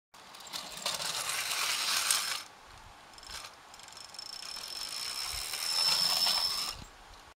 Unexpected Problem
This video features a Hornby No. 1 Special Tank locomotive. If you are not familiar with Hornby clockwork locomotives you may not know that some have a tab underneath that can stop or reverse the loco according to how a special brake/reverse rail is set. This tab shouldn't be triggered when traversing a pair of points (or switch in American terminology), as it is here.